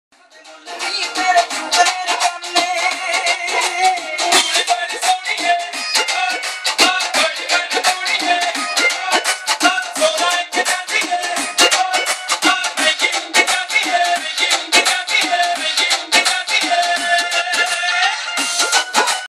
Punjabi Jago - Gordonvale (Australia)
Jasjeet Kalkat & Harvir Hundal Jago @ Farm, with My Setup Ft DJ Jesta & Hot Chilli Media